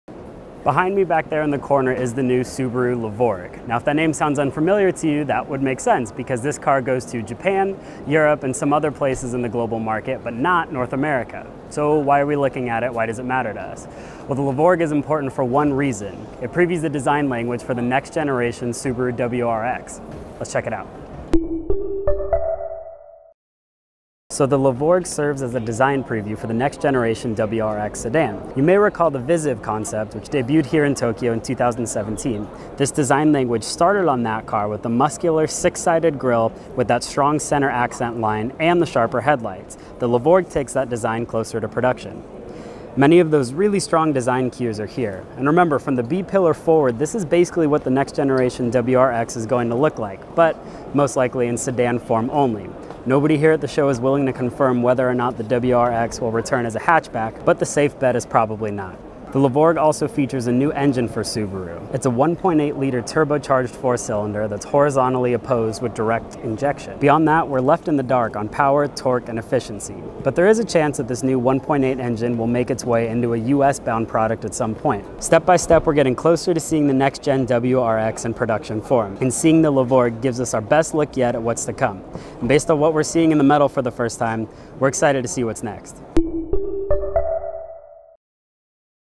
2020 Subaru Levorg Prototype: First Look
The 2020 Subaru Levorg Prototype previews the design language for the next-generation WRX sedan.